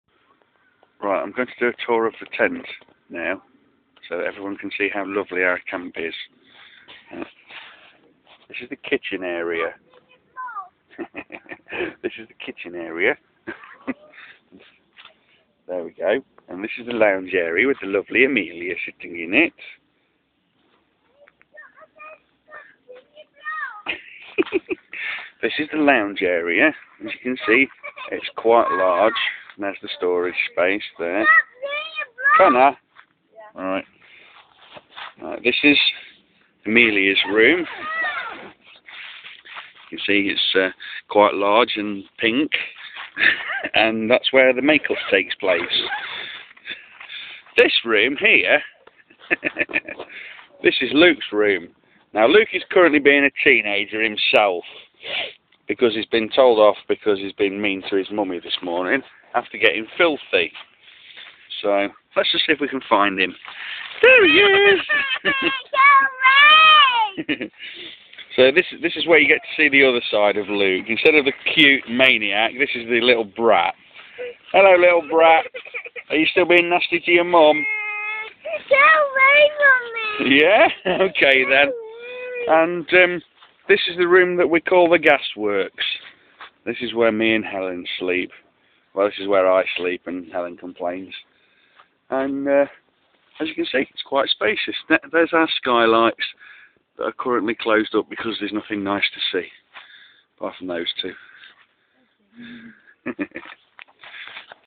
France: Tent Tour!
Just a quick and badly narrated tour of the tent we stayed in, in France. Yeah, the living room's MASSIVE!